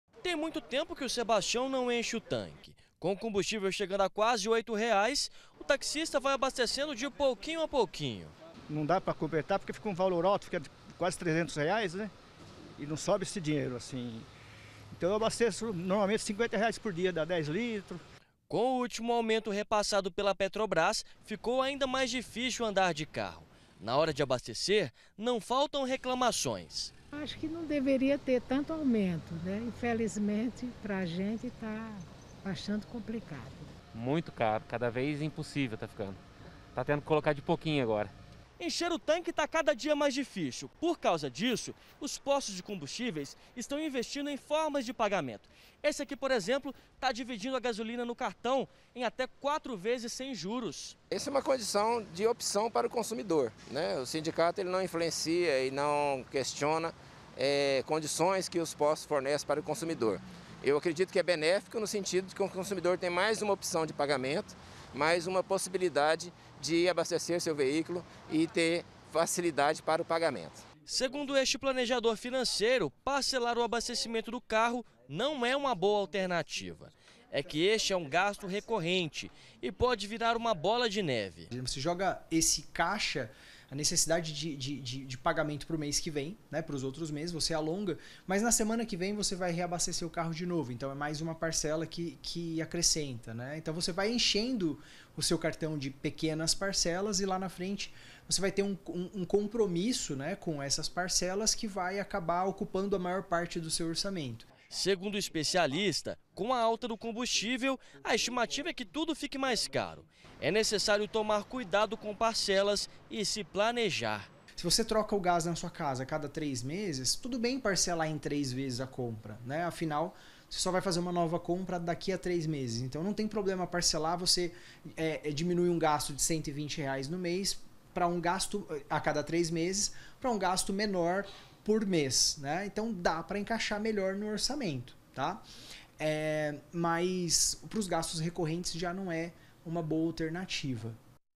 PARCELAMENTO: POSTO DIVIDE GASOLINA EM ATÉ 4X
DESDE QUE O PREÇO DOS COMBUSTÍVEIS DISPARARAM, ENCHER O TANQUE ESTÁ CADA DIA MAIS DIFÍCIL E PRA ATRAIR OS CLIENTES OS POSTOS ESTÃO INVESTINDO EM FORMAS DE PAGAMENTO. TEM POSTO QUE DIVIDE A GASOLINA EM ATÉ QUATRO VEZES NO CARTÃO DE CRÉDITO. MAS SERÁ QUE É UMA BOA SAÍDA?